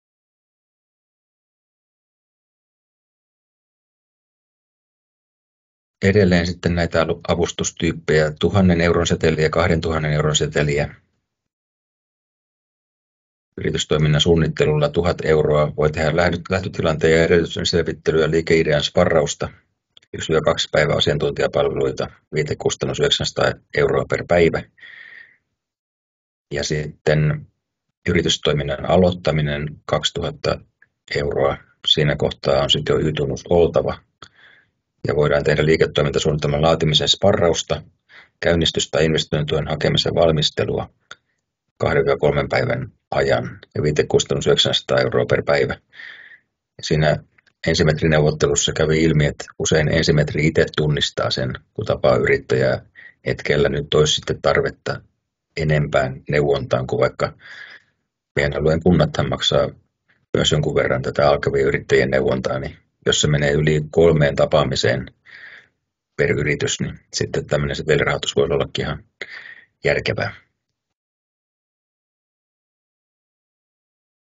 Kehittämisavustukset yrityksen perustamiseen
Satakunnan yritystukia käsittelevässä webinaarissa (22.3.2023) käsiteltiin energia-asioita sekä Leader-rahoituksia. Energia-asioita avasi ProAgrian Länsi-Suomen asiantuntija Manu Hollmen ja Leader-rahoituksia Joutsenten reitin toiminnanjohtaja Petri Rinne.

Webinaarin järjestivät Leader Joutsenten reitti sekä Maaseutuyritysten kasvun portaat ja Energiaälykäs maaseutu -hankkeet.